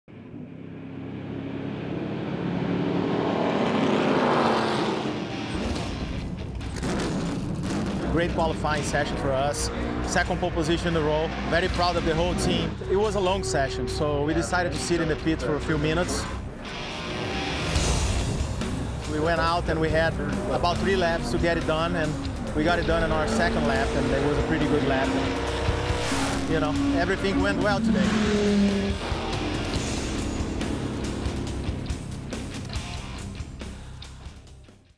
Matos Takes Pole in TA2 at Road America
Rafa Matos captured his third pole of the season in qualifying for the Ryan Companies Muscle Car Challenge featuring the TA2 powered by AEM.

Matos turned a lap of 1:12.459-seconds in the No. 88 3Dimensional Services Group Chevrolet Camaro to beat out Marc Miller and Misha Goikhberg, who both have two pole positions in 2019 TA2 qualifying.